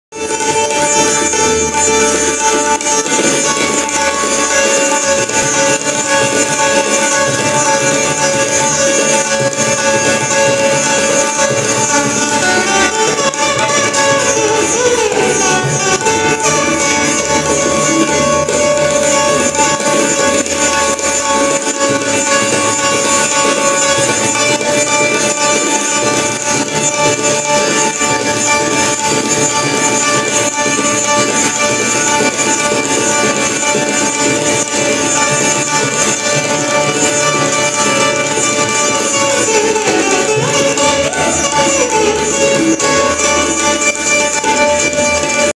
Rahul Kumar Mishra with with Grammy winner pt. vishwamohan bhatt and pt. Ram kumar mishra part2
tabla